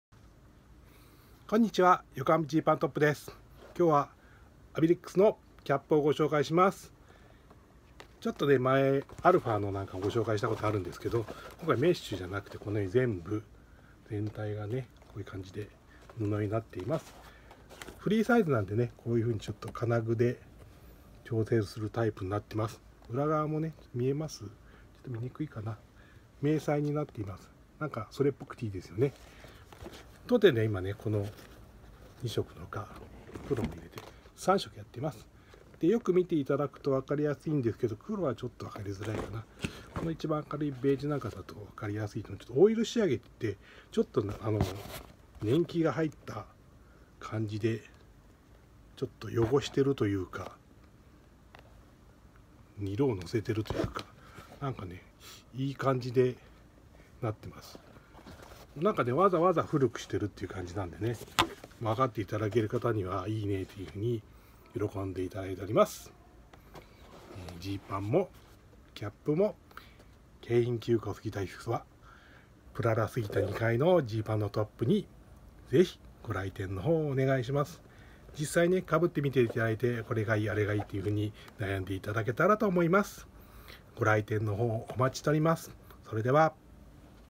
アビリックスのキャップのご紹介 横浜ジーパンのトップ
おかげさまでで完売となりました。ありがとうございます。
アビリックスの新作キャップです！オイル仕上げで年季入って感を出しています。ぜひ お手にとってご覧になってみてくださいね！御来店お待ちしております。